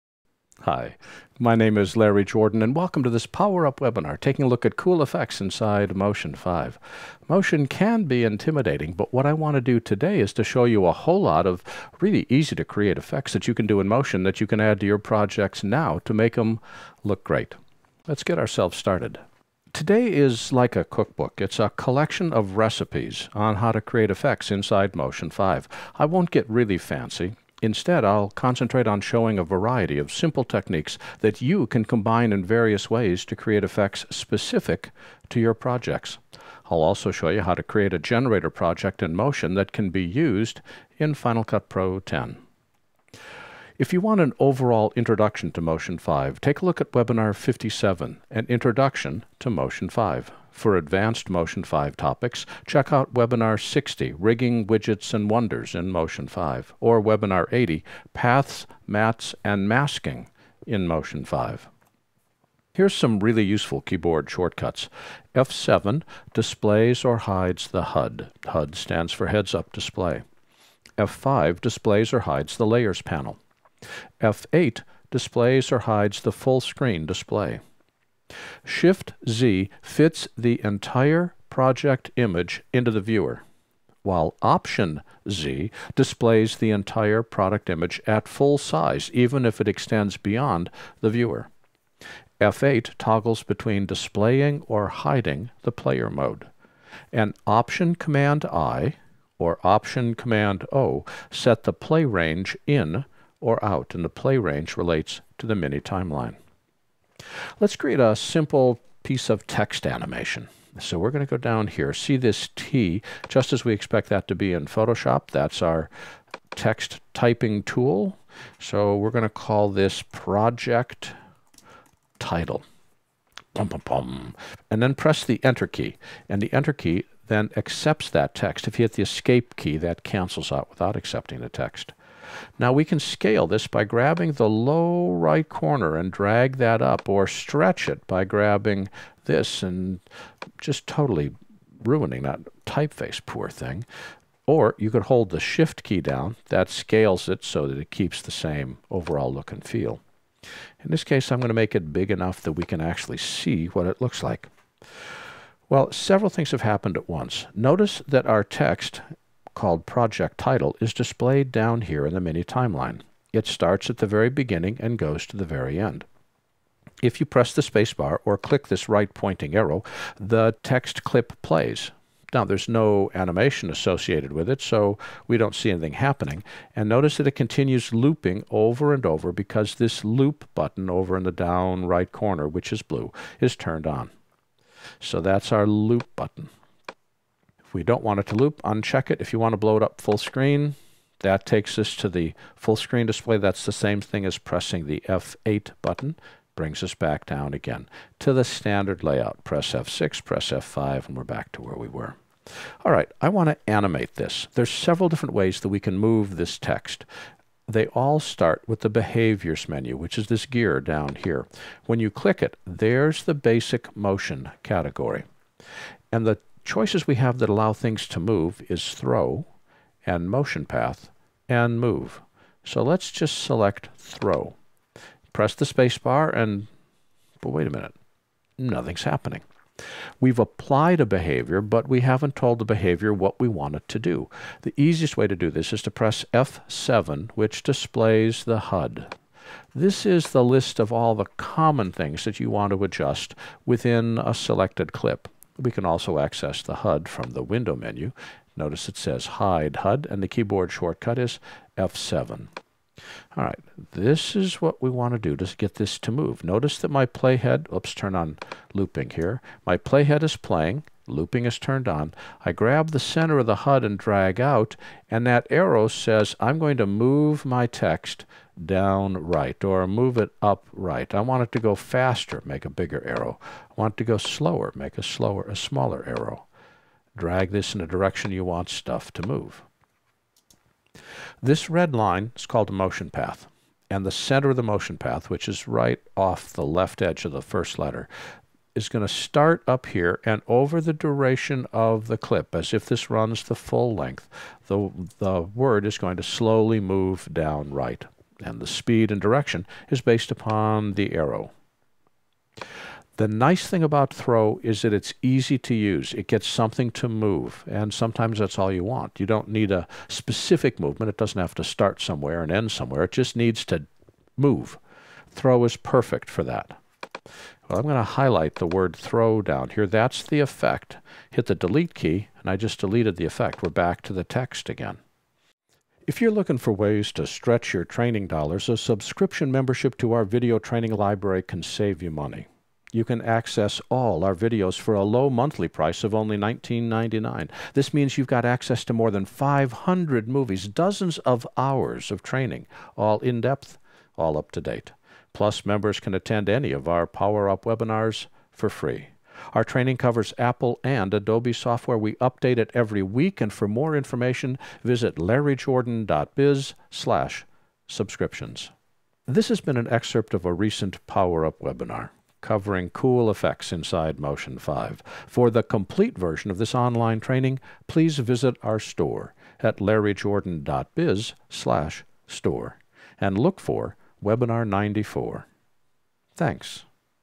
Create Cool Effects in Motion 5 (webinar preview)
- In this Motion 5 webinar preview, Larry Jordan introduces his webinar on how to Create Cool Effects in Motion 5 - Enjoy!